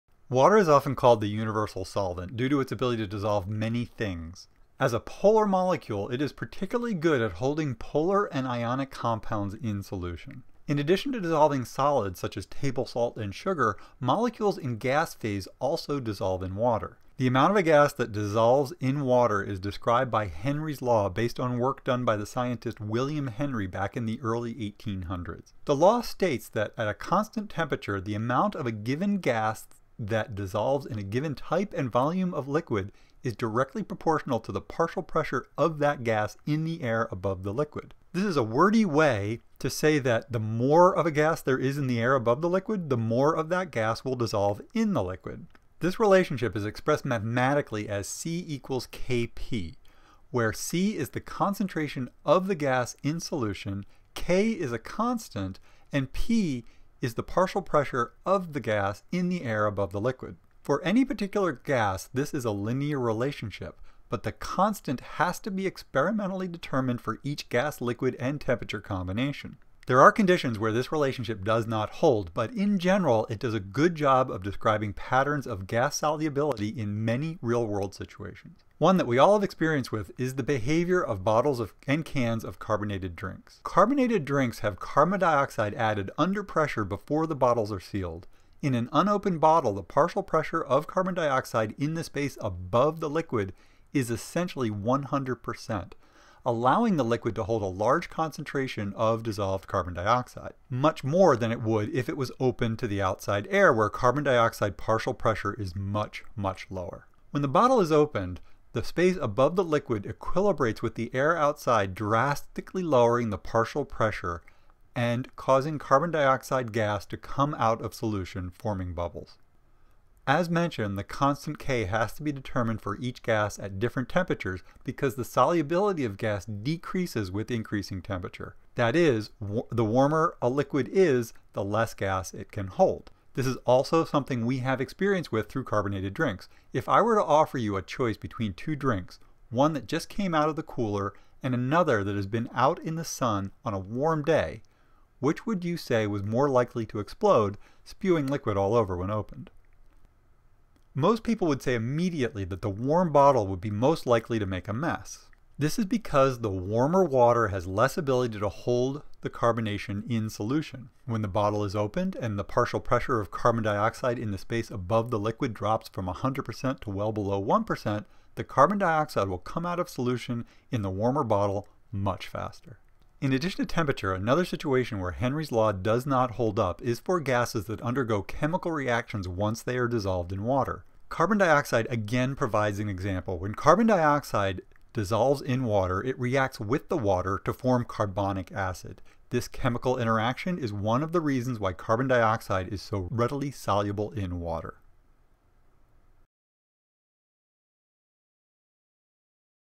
Gas Solubility and Henry's Law
Henry’s Law and gas solubility in water using carbon dioxide as an example gas.